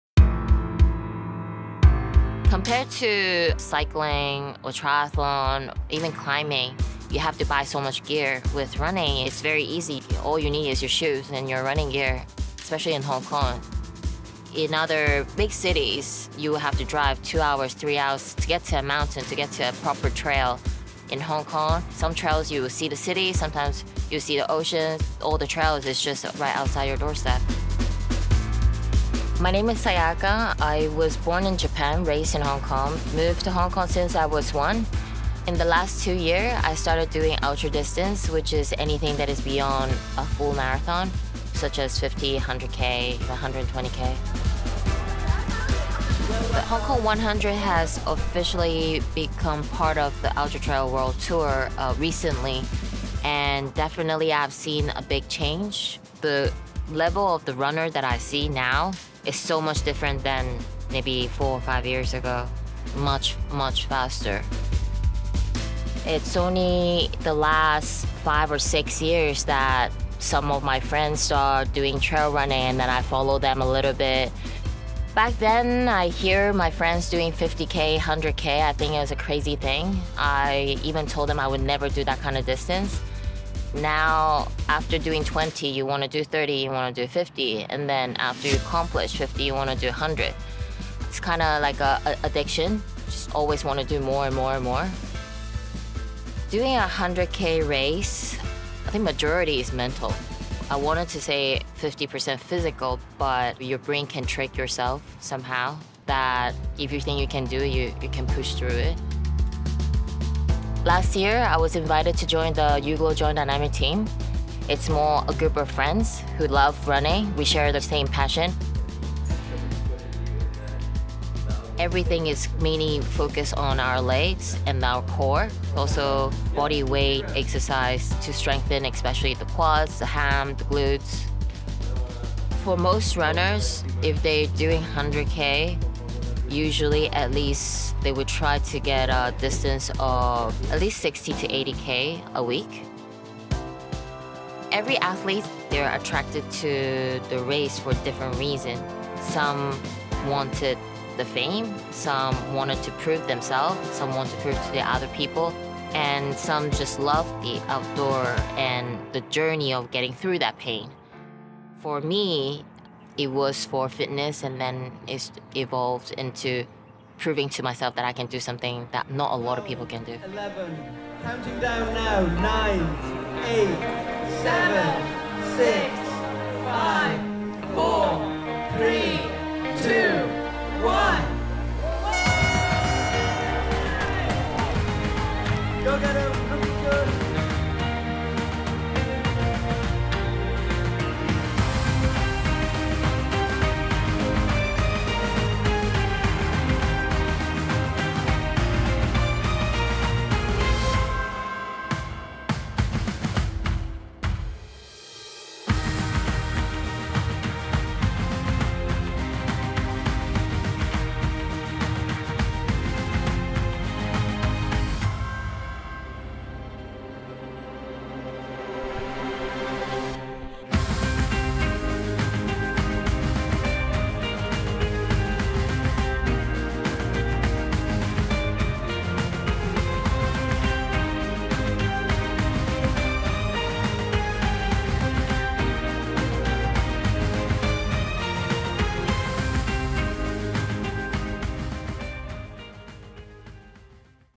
Ultra runner reflects on her journey to running 100k
It’s like an addiction,” says property agent Sayaka Matsumoto who switched from hiking to trail running two years ago. She recently participated in the HK100 with the elevation gains of 5,300 metres (17,400 feet), 14 times the height of New York’s 102-storey Empire State Building, throughout the 103km route.